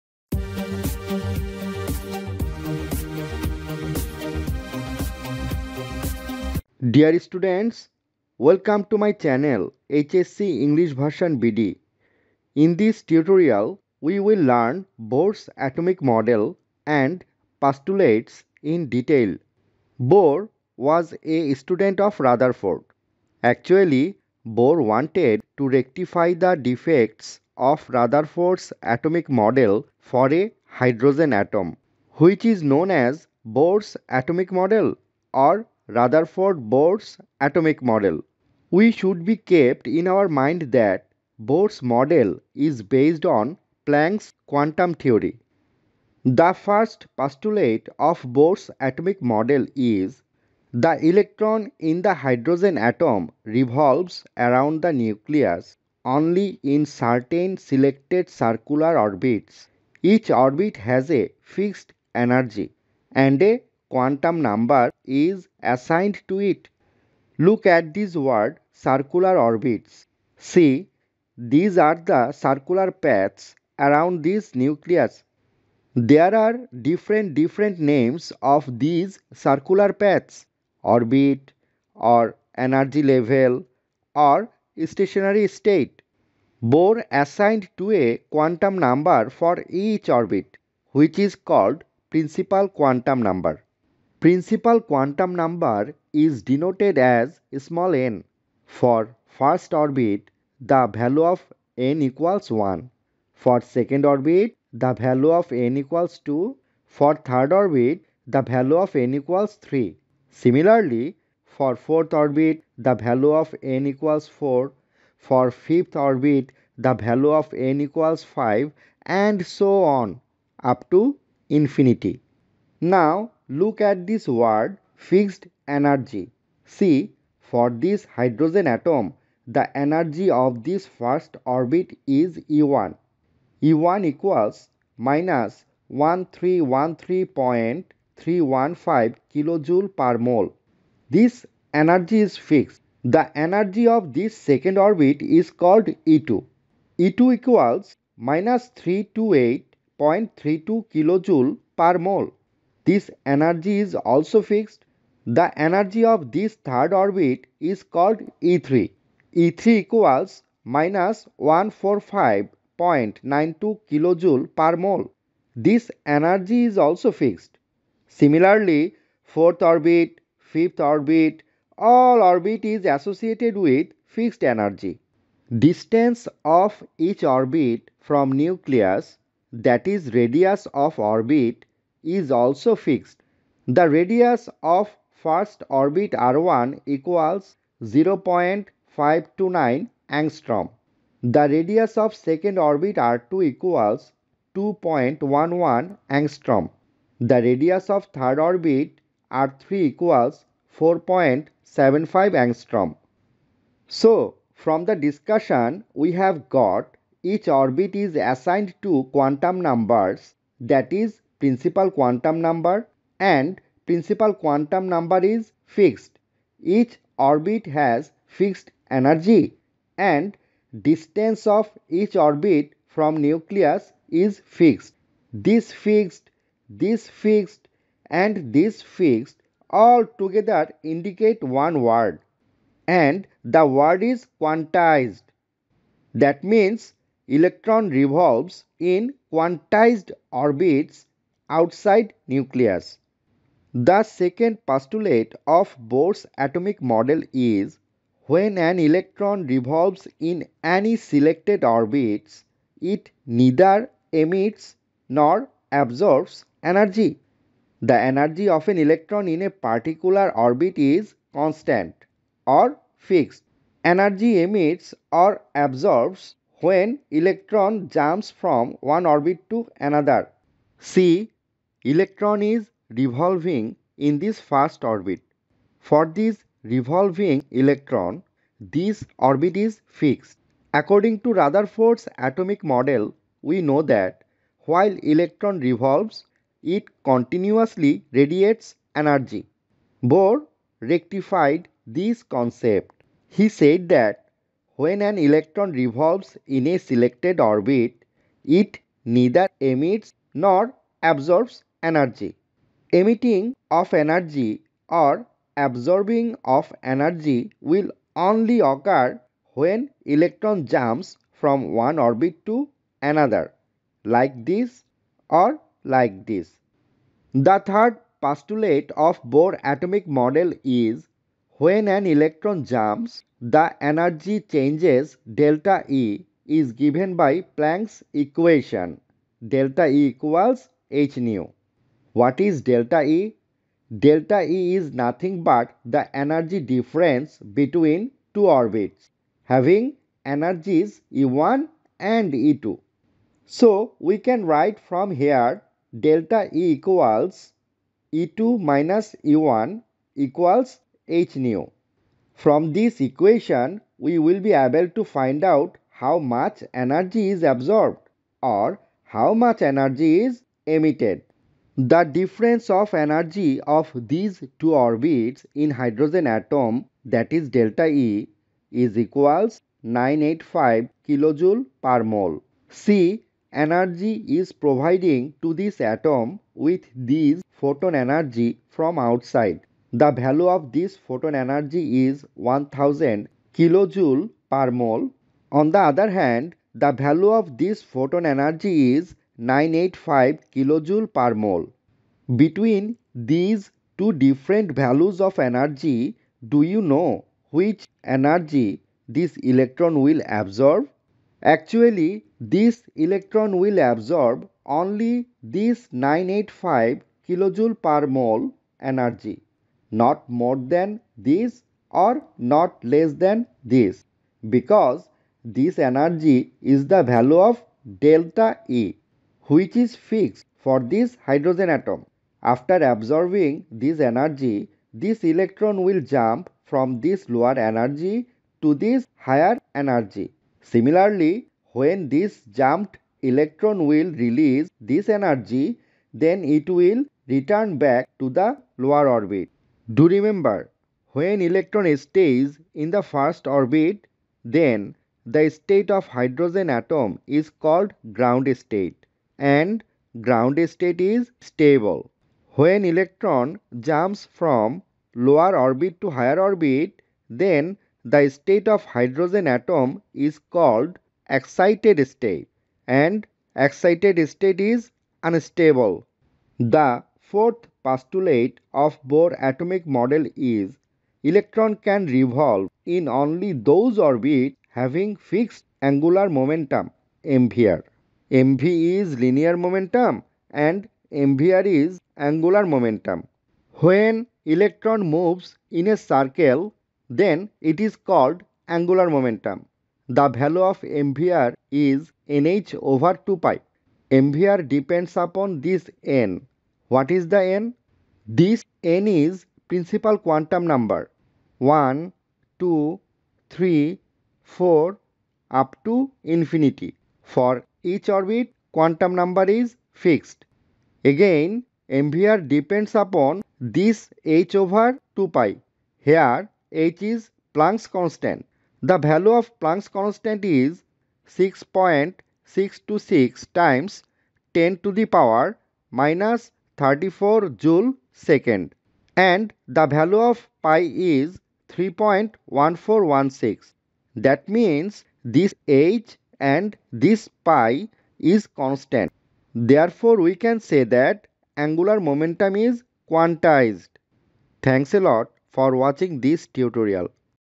03. HSC Chemistry 1st Paper ||English Version || Chapter 2 || Bohr's Atomic Model & Postulates
The Students who wish to understand any lesson of Chemistry, Physics, and, Biology with some super tricky way, just stay with me.

Here are all the video tutorial links.